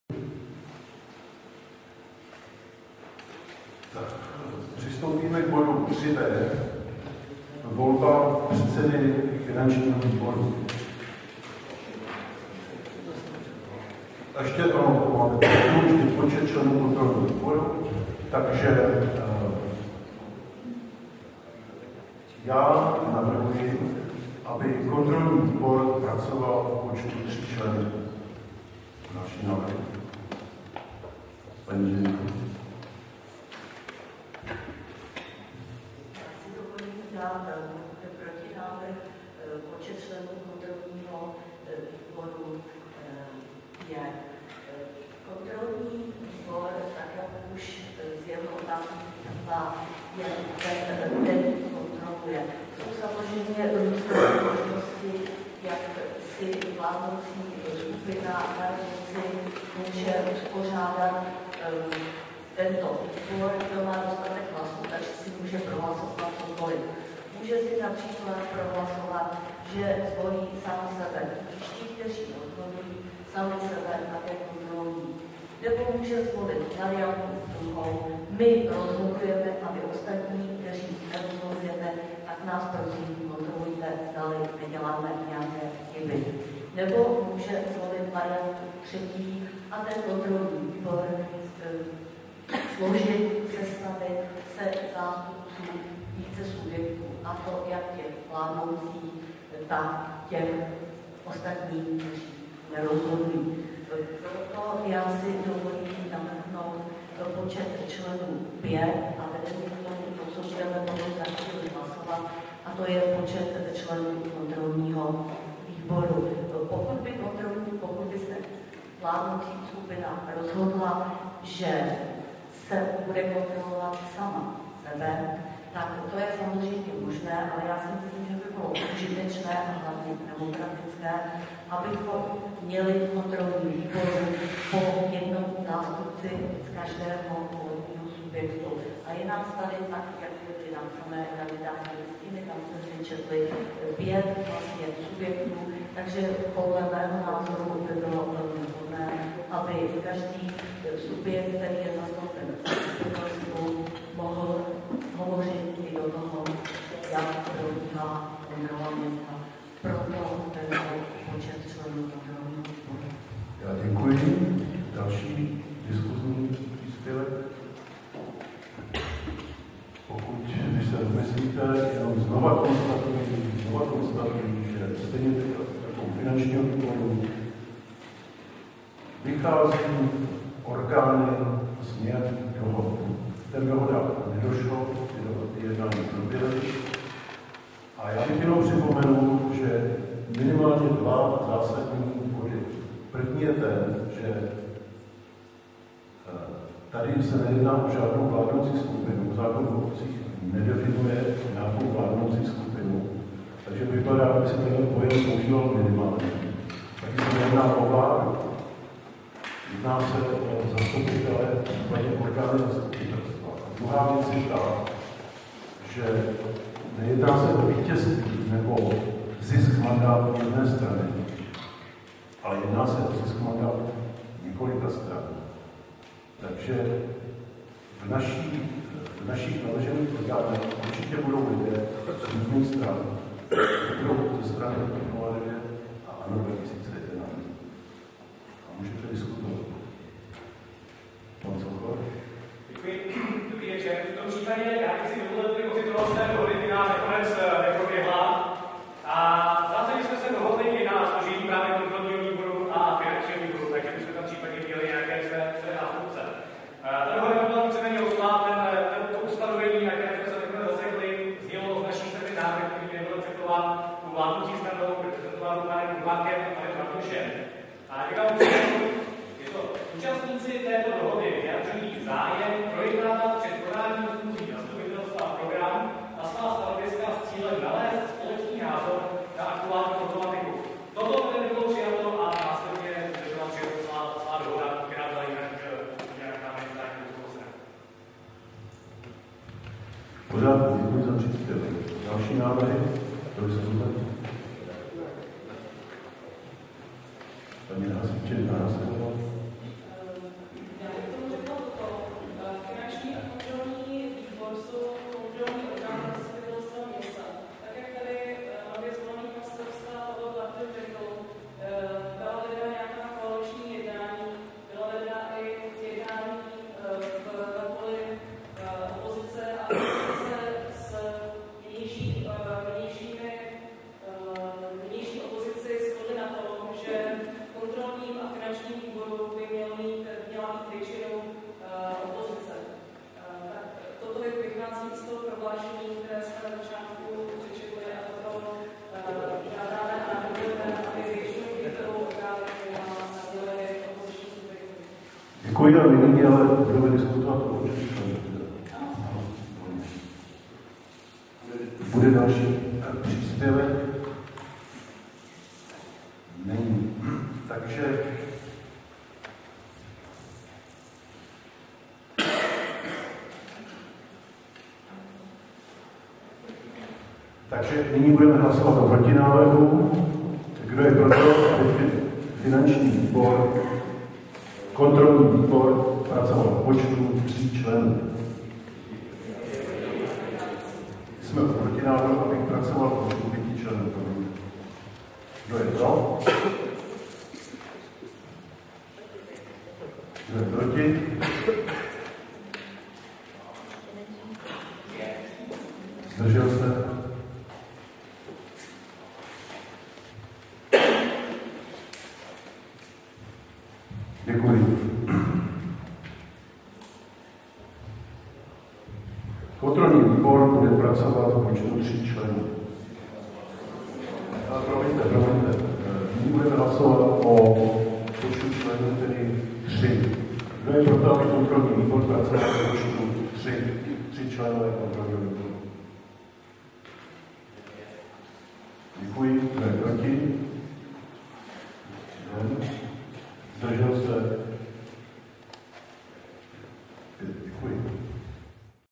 14 Počet členů kontrolního výboru
Záznam z jednání ustavujícího ZM Libčice nad Vlt, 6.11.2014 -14 Počet členů kontrolního výboru